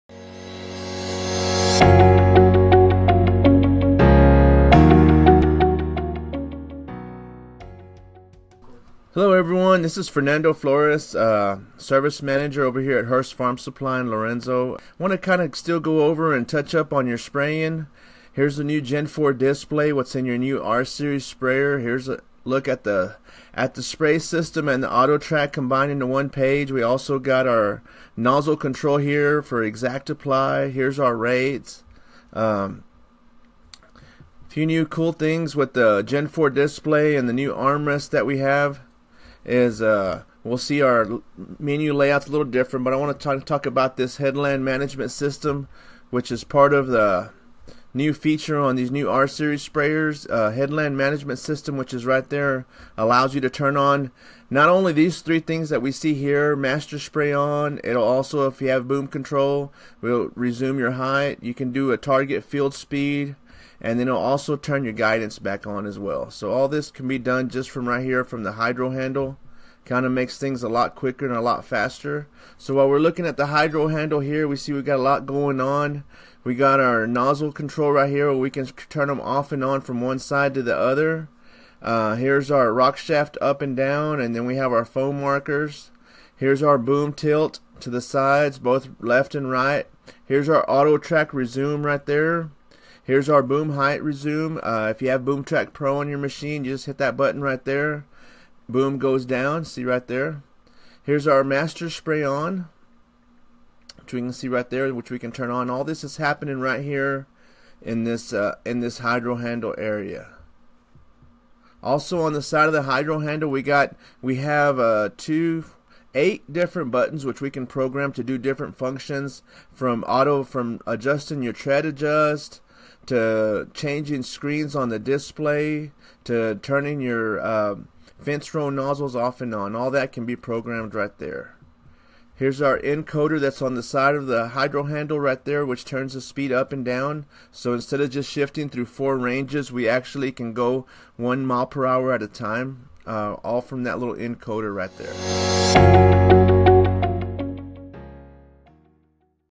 R Series Sprayers - Headland Management System and Hydro Handle Overview
In this video we talk about the headland management system that is available on MY18+ R-Series sprayers. We also go over the redesigned hydro handle.